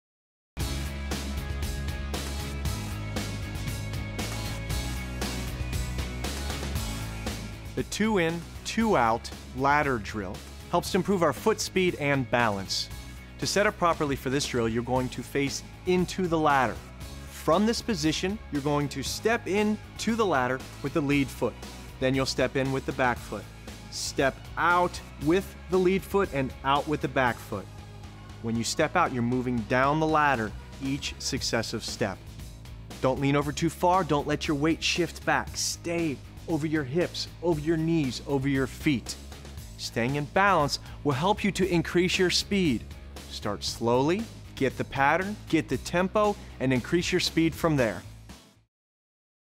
2 IN 2 OUT LADDER DRILL
The 2 In 2 Out Ladder Drill helps to improve foot speed and balance.